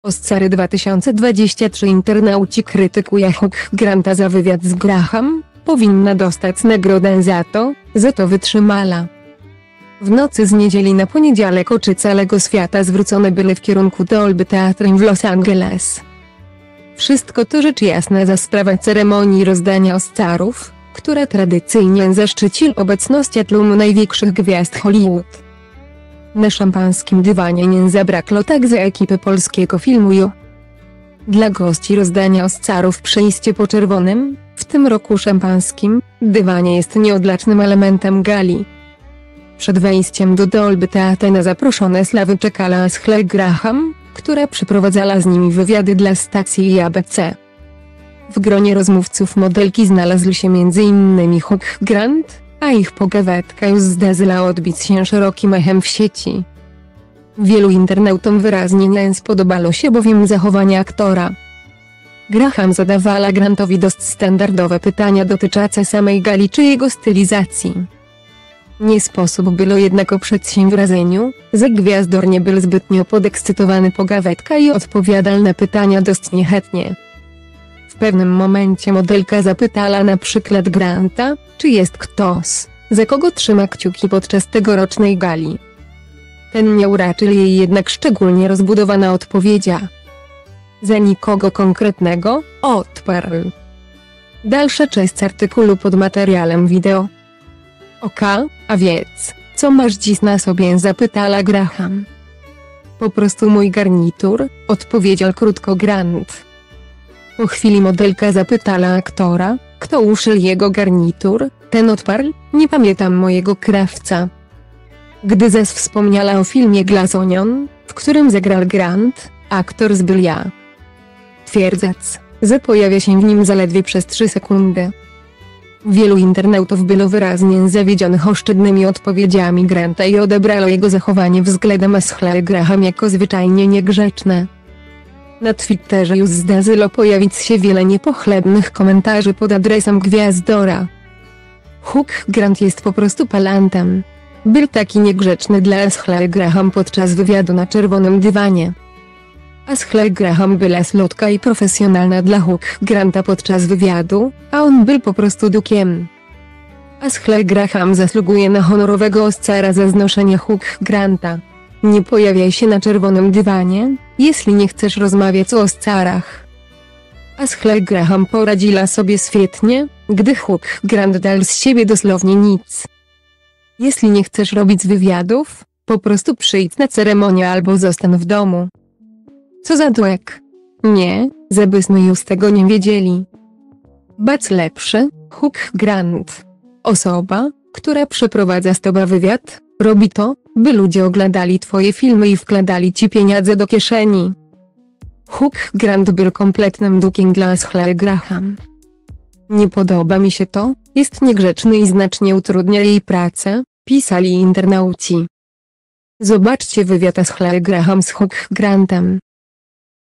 Oscary 2023.Internauci krytykuja Hugh Granta za wywiad z Graham."Powinna dostac nagrode za to, ze t
Oscary 2023.Internauci krytykuja Hugh Granta za wywiad z  Graham."Powinna dostac nagrode za to, ze to wytrzymala"
W nocy z niedzieli na poniedzialek oczy calego swiata zwrócone byly w kierunku Dolby Theatre w Los Angeles.Wszystko to rzecz jasna za sprawa ceremonii rozdania Oscarów, która tradycyjnie zaszczycil obecnoscia tlum najwiekszych gwiazd Hollywood.Na szampanskim dywanie nie zabraklo takze ekipy polskiego filmu "IO".